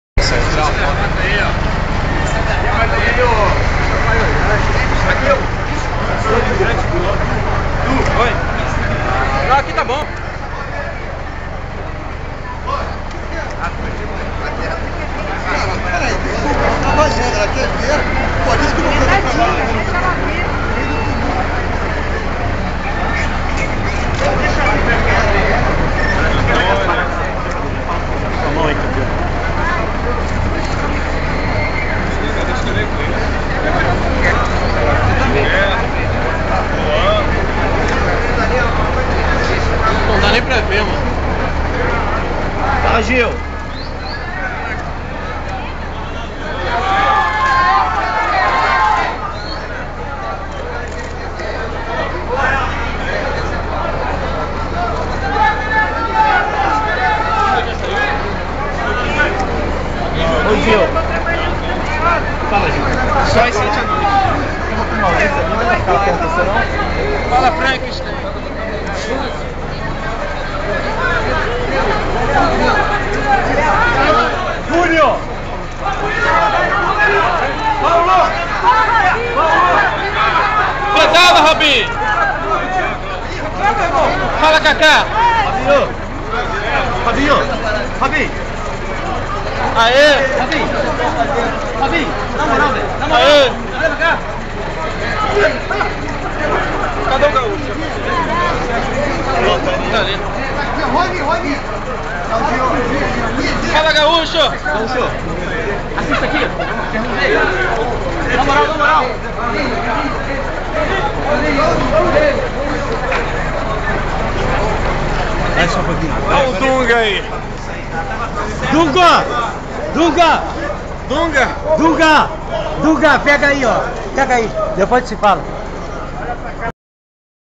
A SELEÇÃO CHEGANDO PARA TREINAR NO MARACA
A SELEÇÃO DE DUNGA E COMPANHIA CHEGANDO PARA TREINAR NO MARACANÃ JOGO CONTRA O ECUADOR.